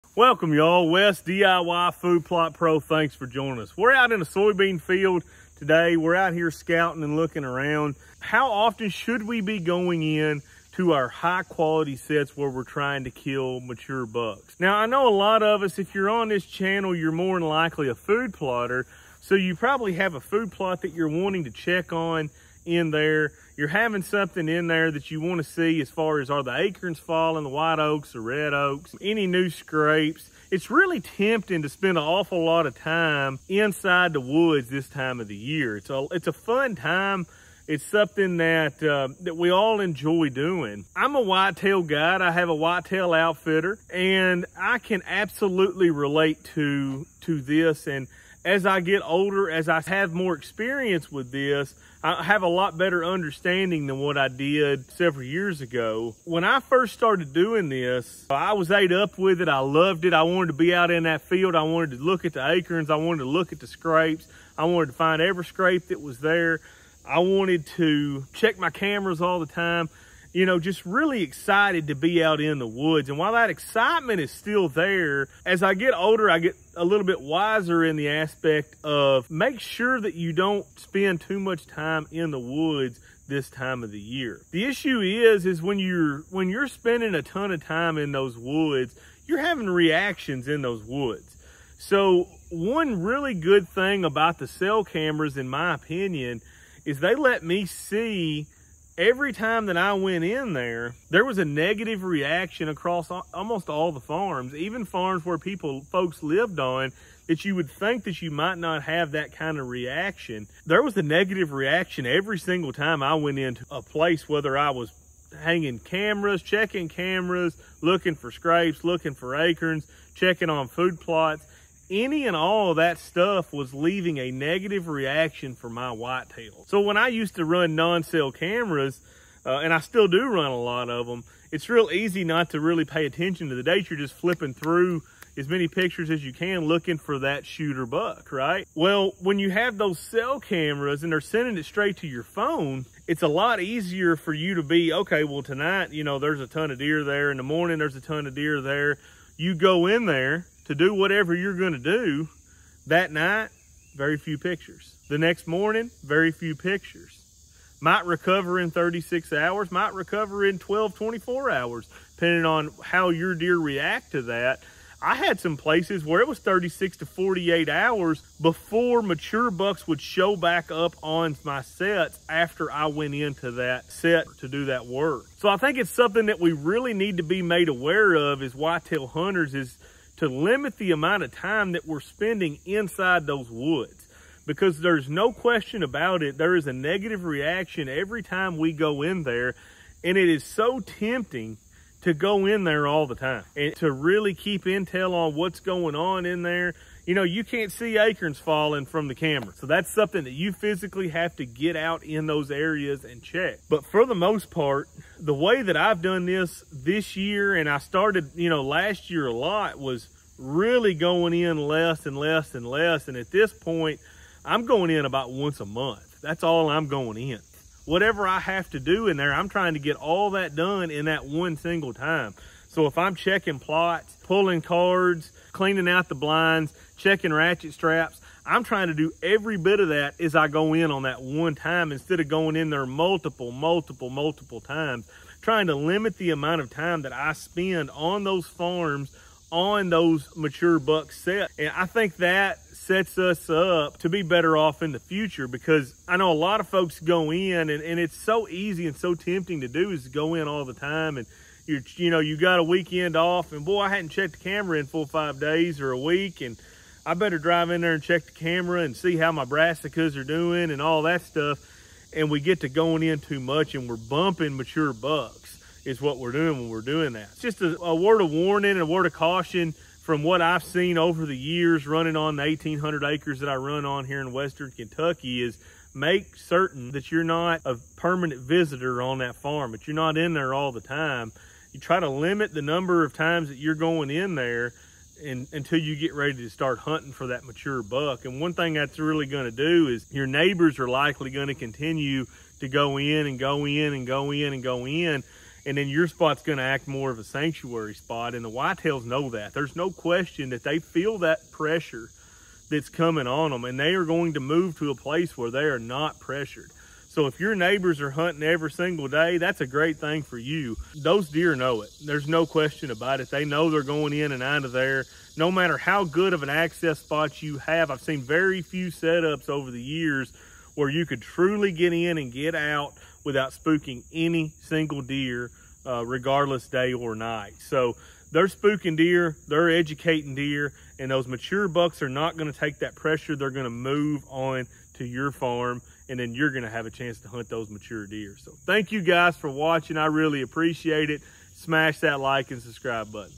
#1 REASON MATURE BUCKS ARE LEAVING YOUR FARM
In this video I discuss why mature whitetail bucks leave farms because of pressure caused by hunters going into the deer hunting areas to often. Checking acorns, food plots, stands, blinds, scrapes, rubs, and checking cameras are all reasons that contribute to us spending to much time on the farms and education mature whitetail bucks that we are in the woods coming after them.